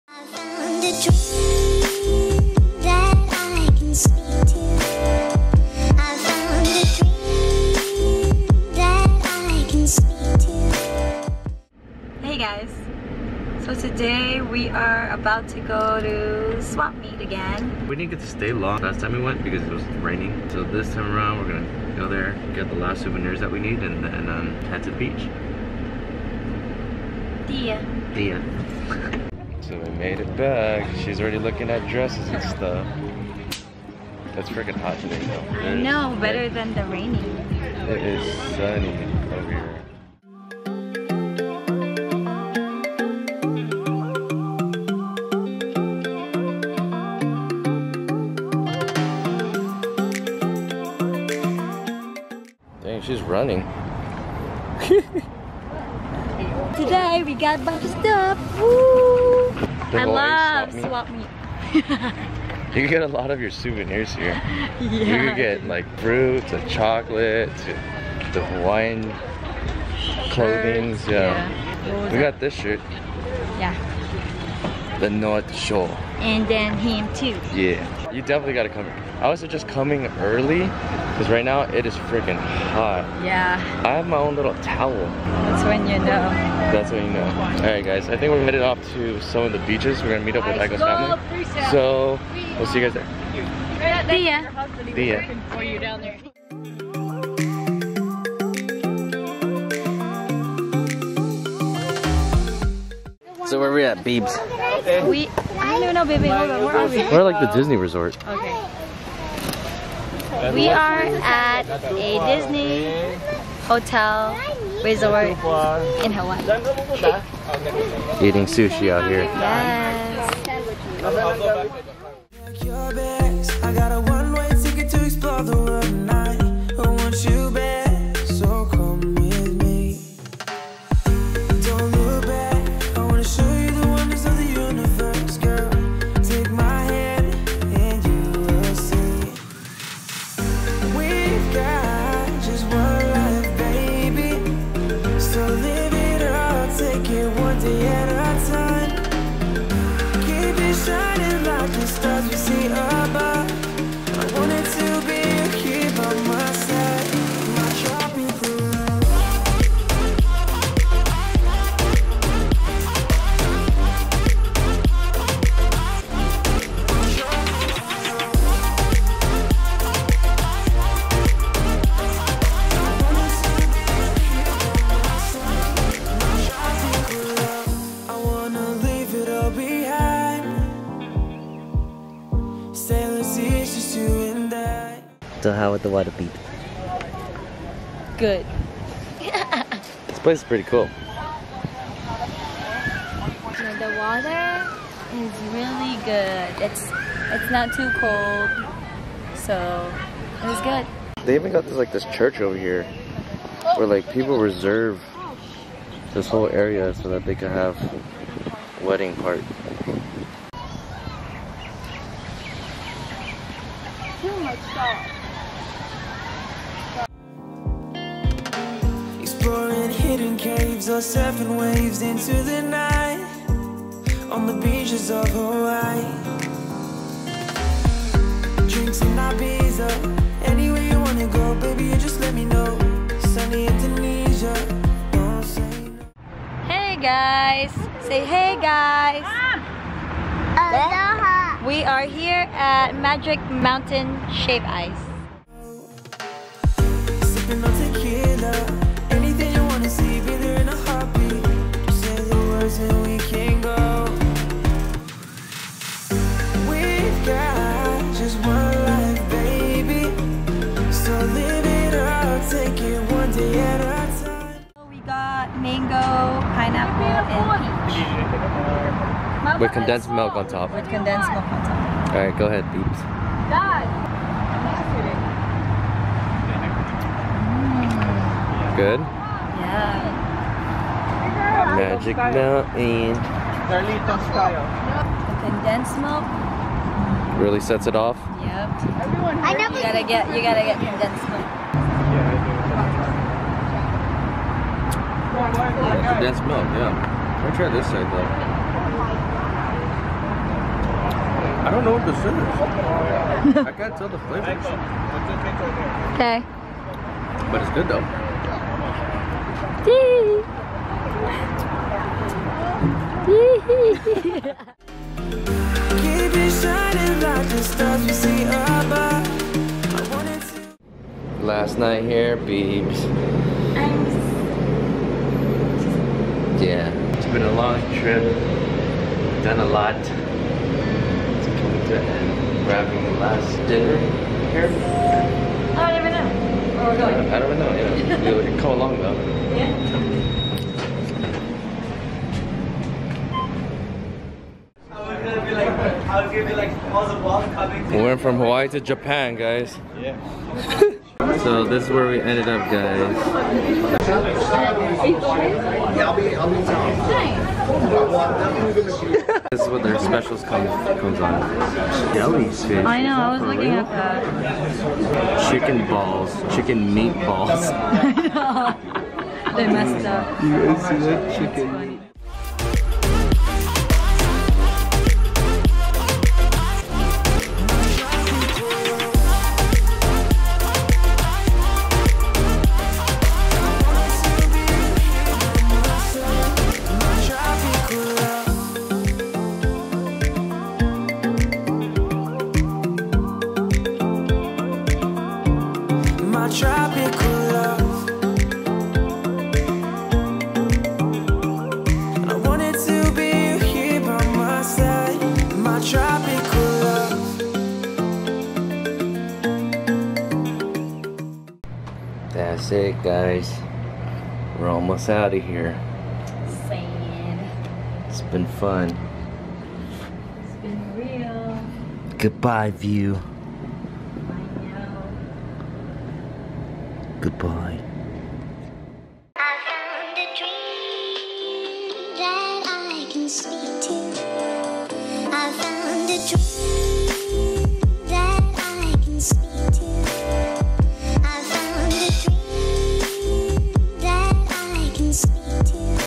EXPLORING OAHU, HAWAII 2019! DAY 6!
Our final last day spend on the beautiful island Oahu of Hawaii...

Revisted Aloha Stadium Swap Meet again to make use of better weather (0:29)
Met up with AIko's family and had a swim at Aulani Disney Resort (1:44)
Stop by Mountain Magic Shave Ice for a snack (3:56)
And ended the night by having dinner at Izakya Pau Hana Base (5:44)

Camera:
Canon G7X Mark II
Canon M50
GoPro Hero 3/3+ Silver